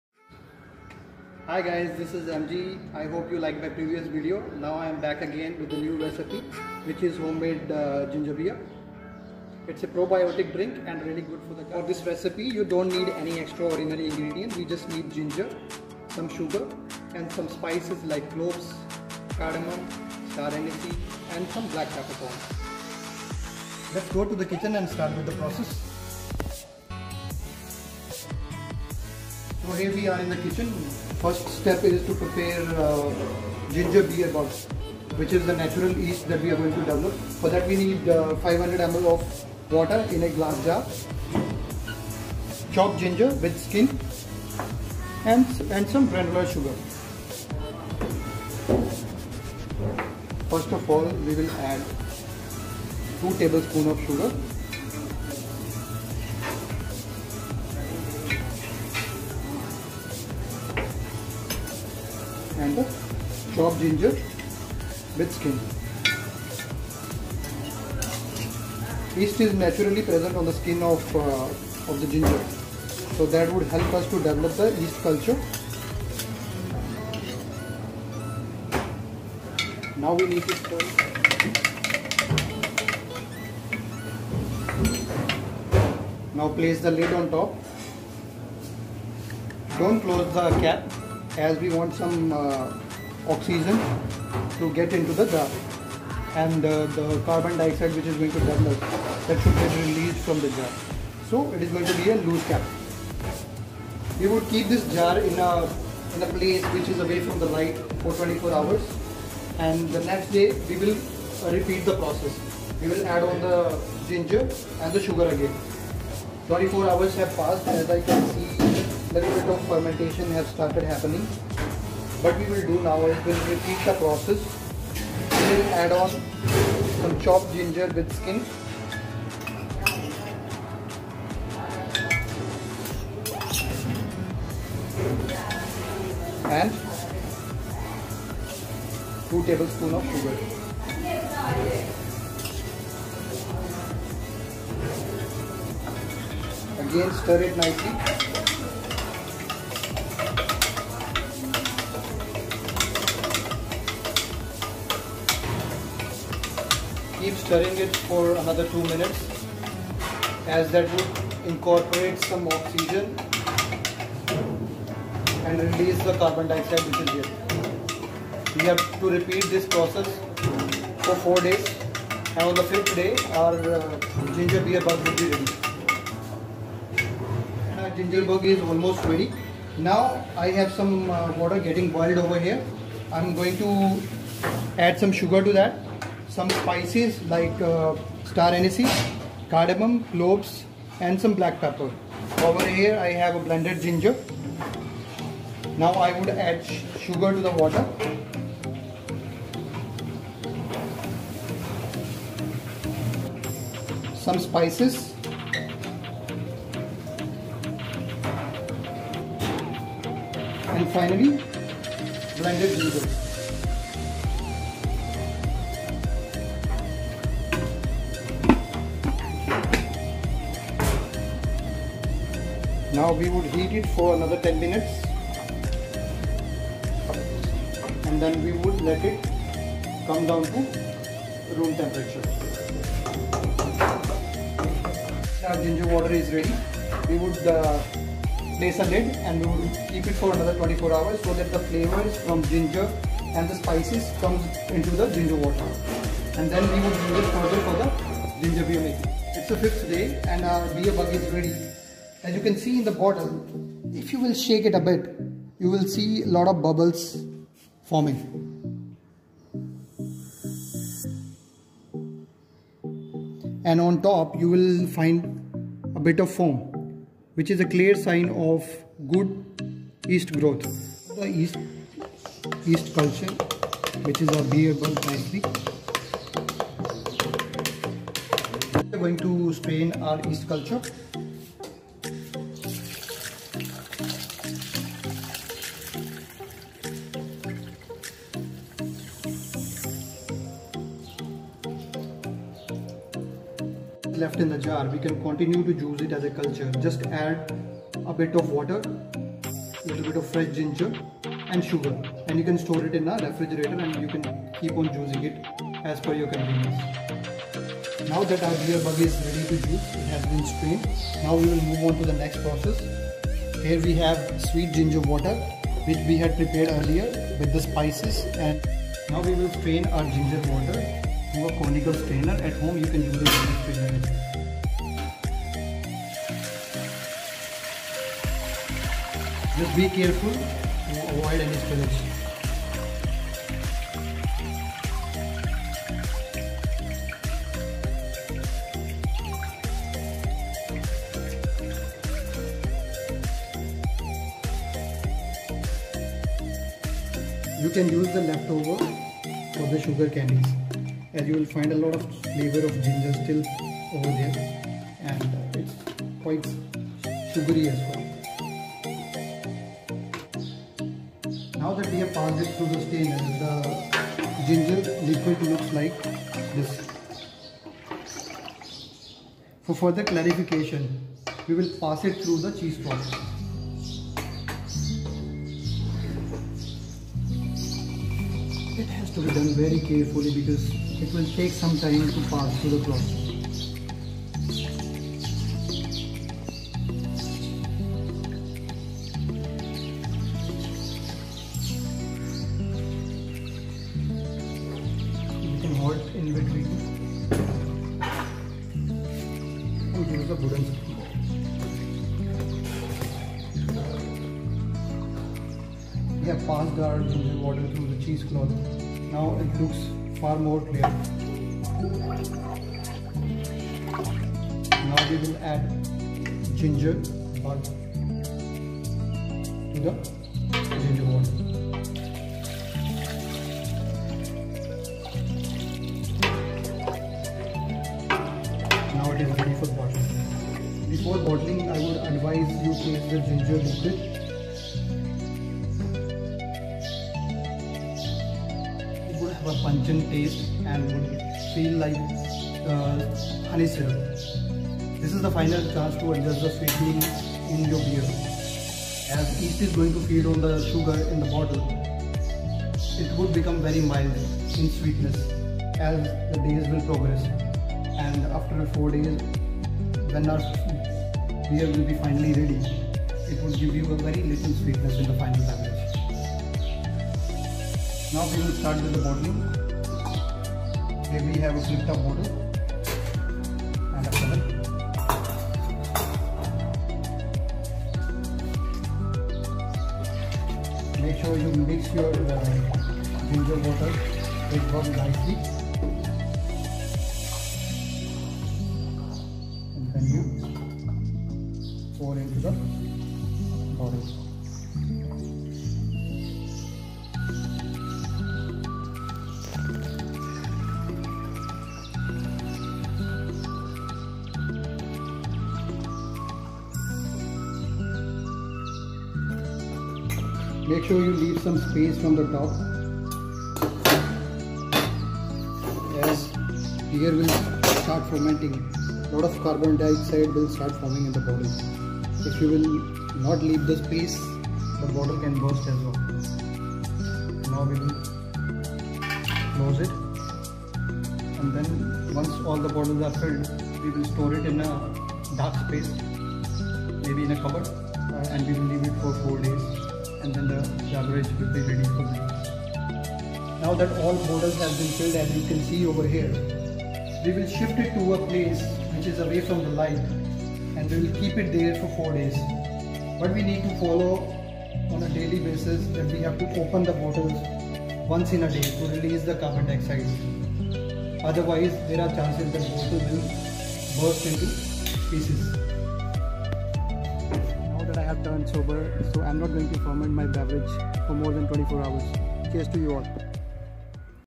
Homemade Ginger Beer
It’s a recipe for making Ginger Beer at home, nothing extra ordinary required for the recipe.

INGREDIENTS:
GINGER – 1 KG
SUGAR – 1.2 KG
WATER – 10 L
CLOVES – 10
STAR ANISE – 05
BLACK PEPPERCORN – 10

To add complexity to the beer you can also add zest of lime and orange, coriander and licorice.

I have prepared my Ginger beer in bulk, you can do the mathematics to produce the desired quantity.

Cheers!

MG